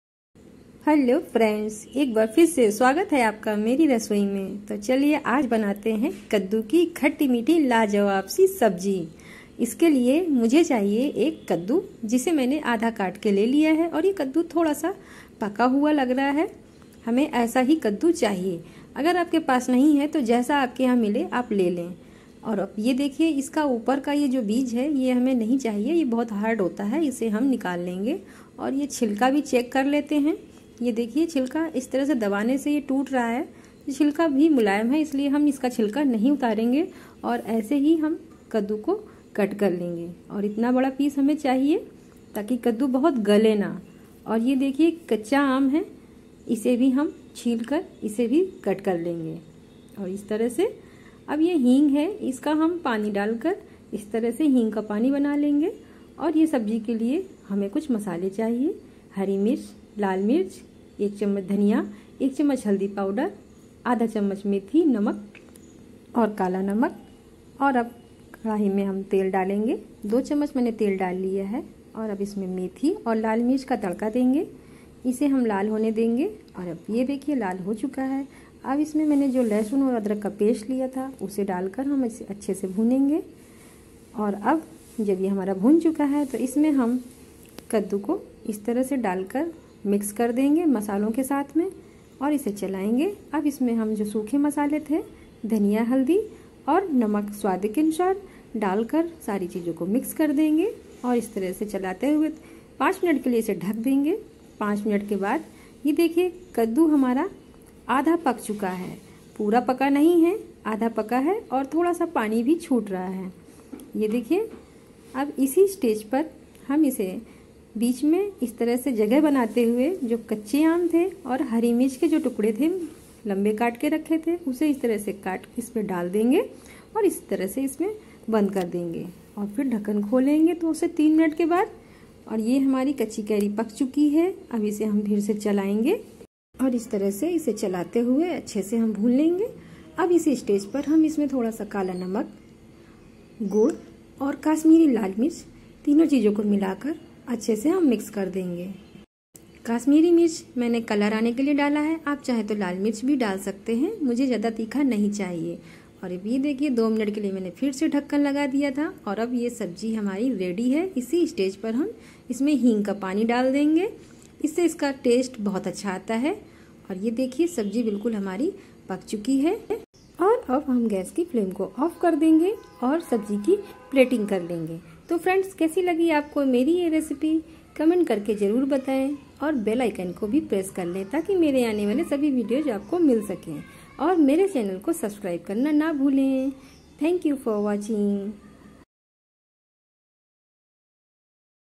Kaddu ki sabji | कद्दू की सब्जी | Mala Singh
कद्दू की सब्जी
कद्दू की खट्टी मीीठी सब्जी
कद्दू की लाजवाब सब्जी जिसेेेेे सब मांग मांग कर खाएंगे,
इस तरह से बनाई गई कद्दू की  सब्जी बहुत ही स्वादिष्ट बनती है।